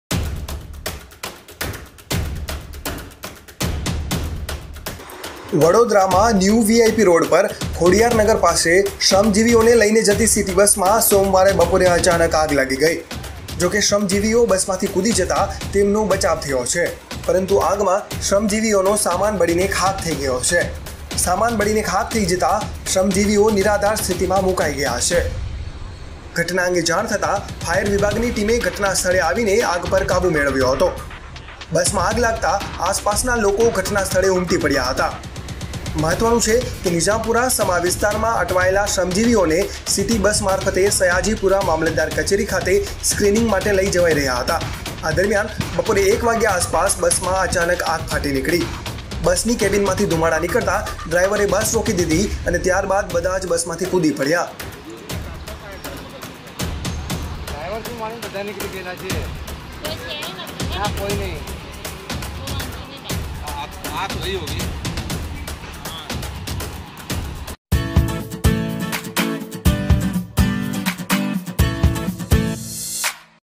વડોદરા: શ્રમજીવીઓને લઈને જતી બસમાં લાગી ગઈ આગ
વડોદરાના ખોડિયારનગર પાસે શ્રમજીવીઓને લઈને જતી બસમાં અચાનક આગ ફાટી નીકળી. બસમાંથી કૂદી જતાં શ્રમજીવીઓનો થયો બચાવ.